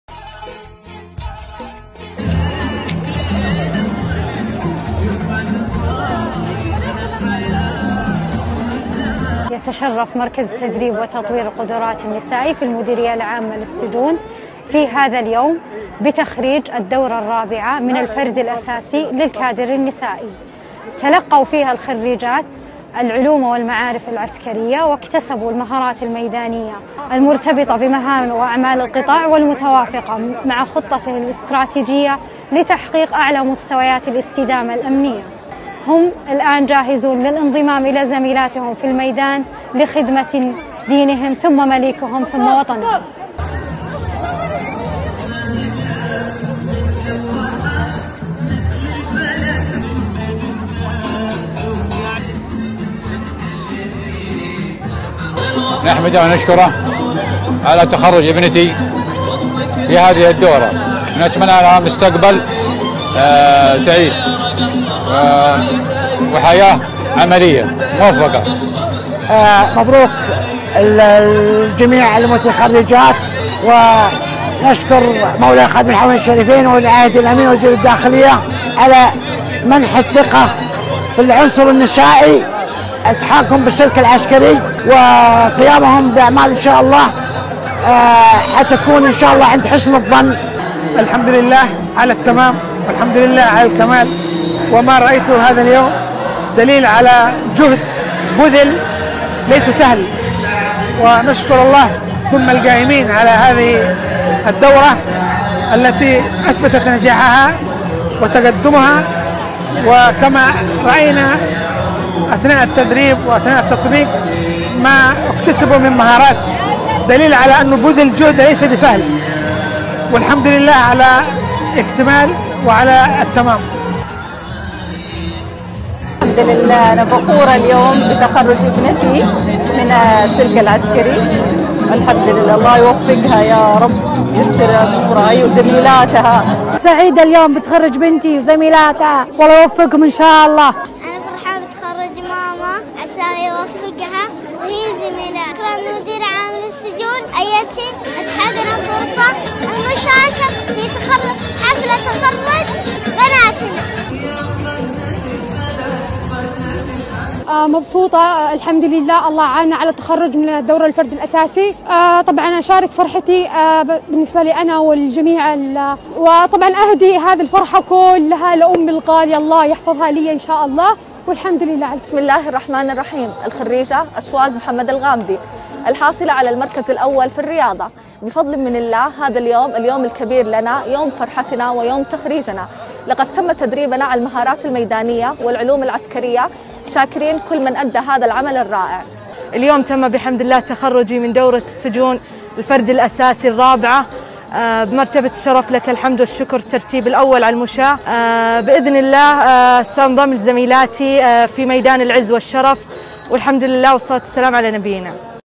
‏لحظات فرح وفخر..‏خريجات المديرية العامة للسجون يحتفلن بتخرجهن، وسط مشاعر من البهجة والسرور.‏-
‏لحظات فرح وفخر..

‏خريجات المديرية العامة للسجون يحتفلن بتخرجهن، وسط مشاعر من البهجة والسرور.